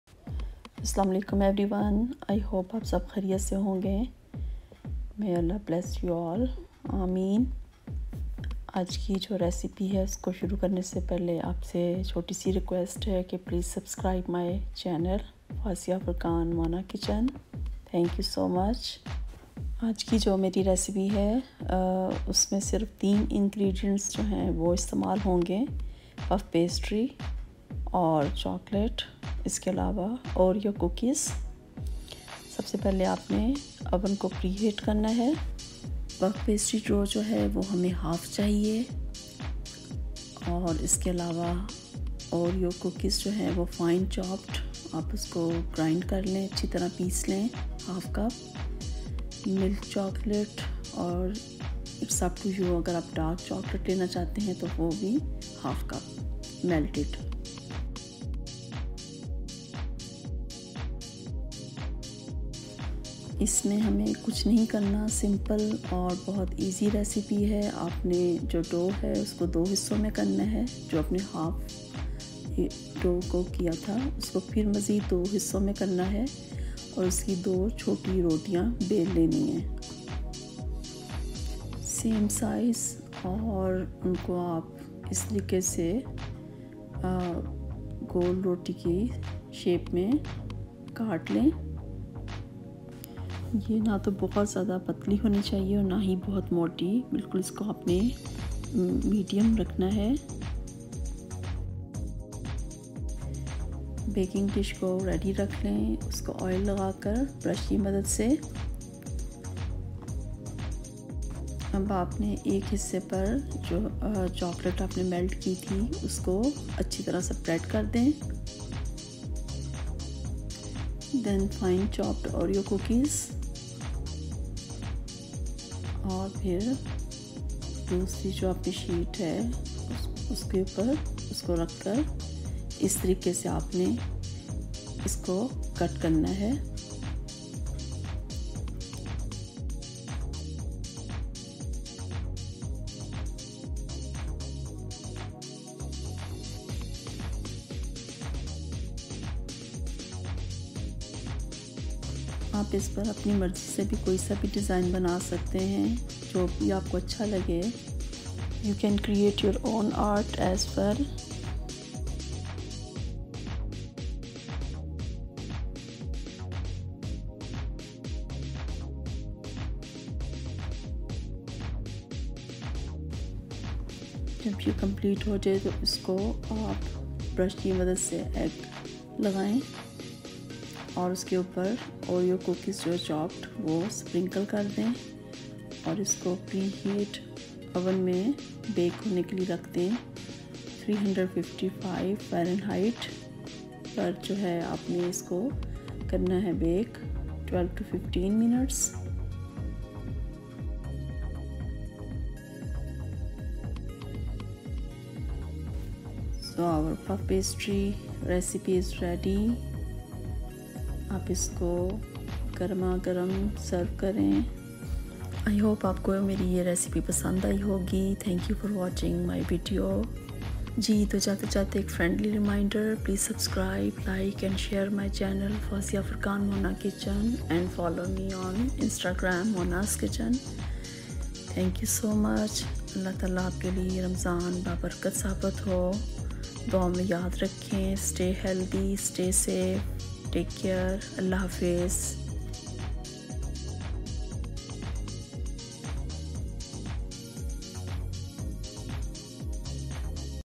Have you tried this? Puff pastry with Oreo and chocolate Only 3 ingredients quick recipe
Ingredients
Puff pastry dough 1/2 sheet
Milk or dark chocolate 1/2 cup
Oreo cookies fine chopped 1/2 cup